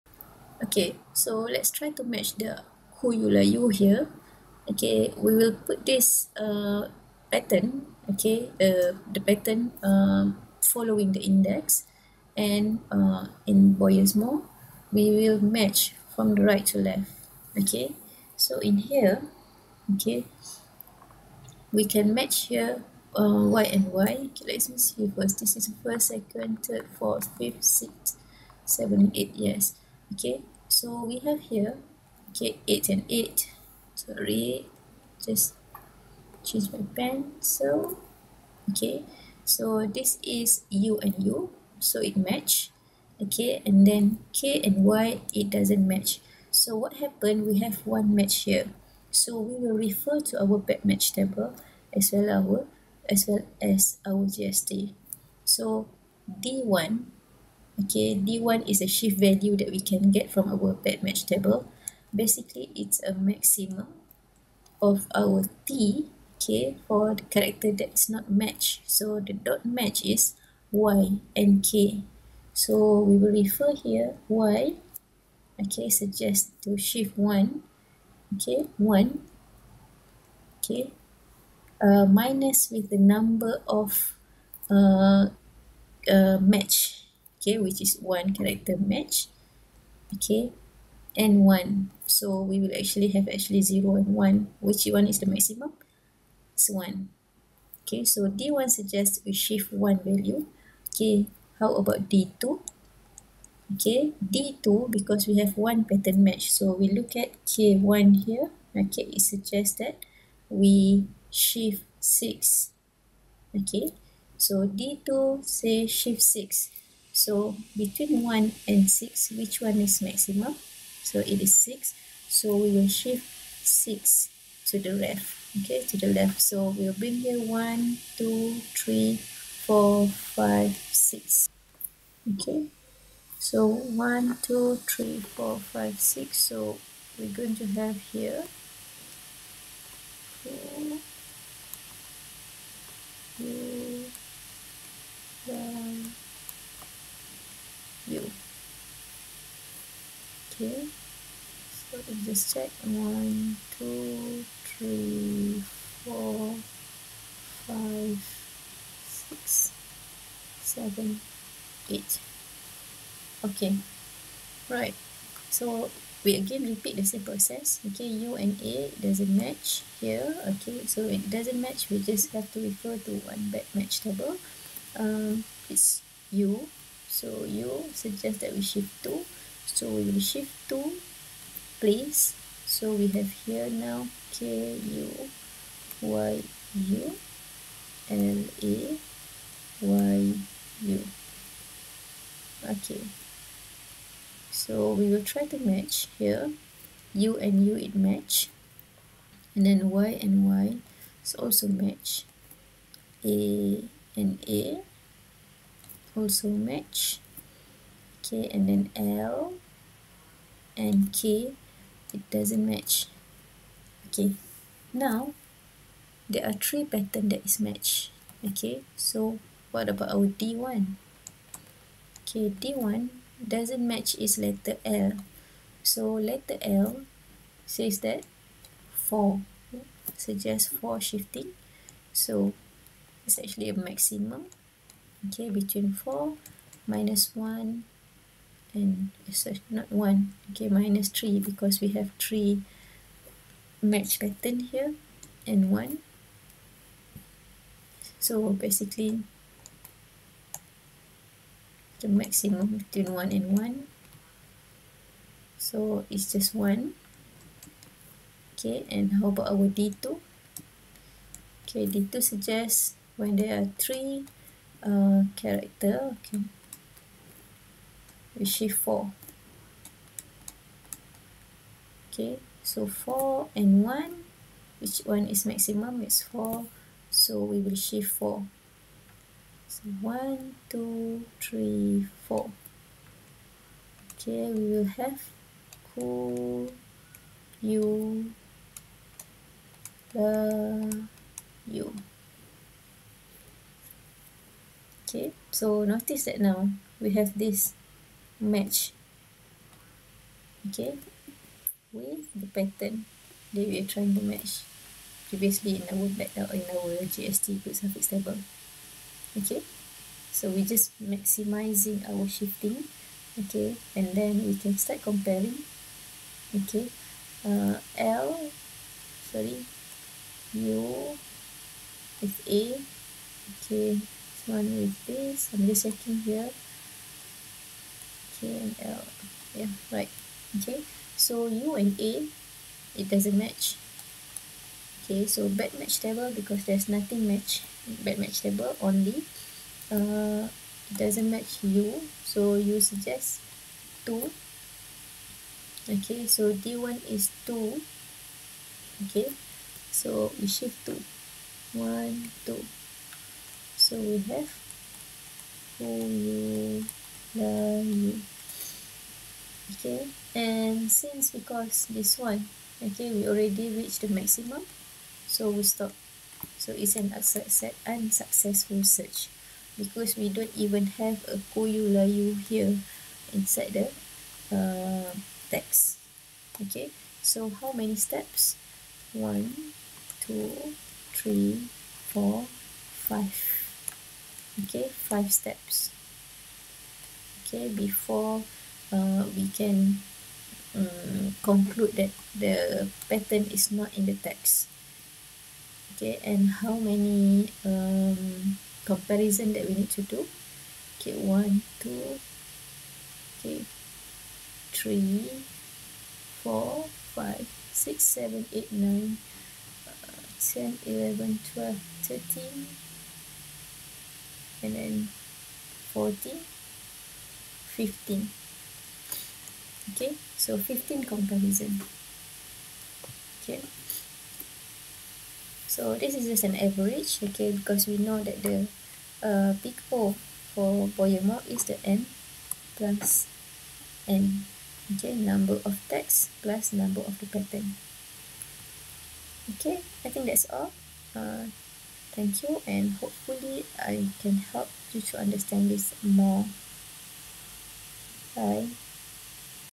Boyer Moore part 2: The matching in process
This is part of video lecture of the above subject from FTSM, UKM